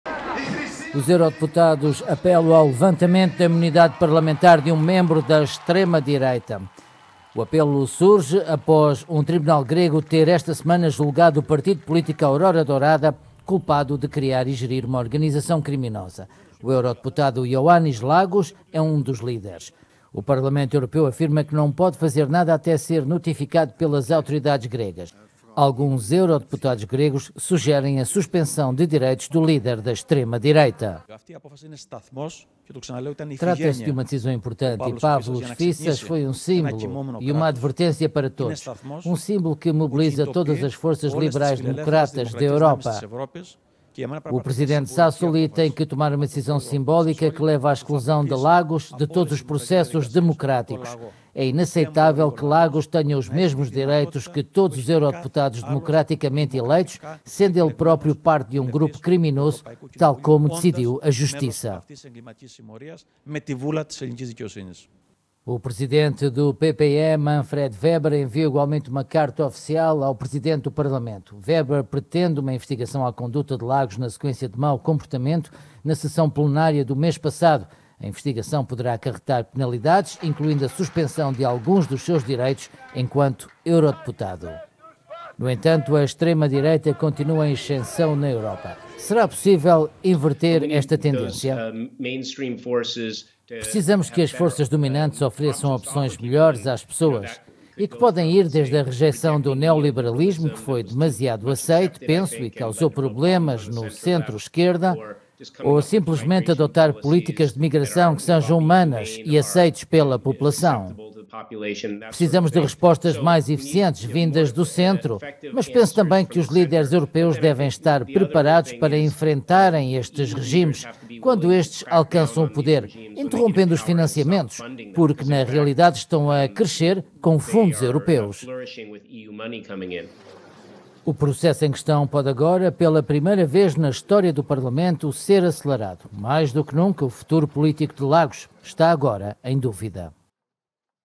Eurodeputados querem afastamento de líder da extrema-direita grega
Parlamento Europeu só poderá levantar a imunidade ao líder do Aurora Dourada depois de ser notificado para tal pelas autoridades gregas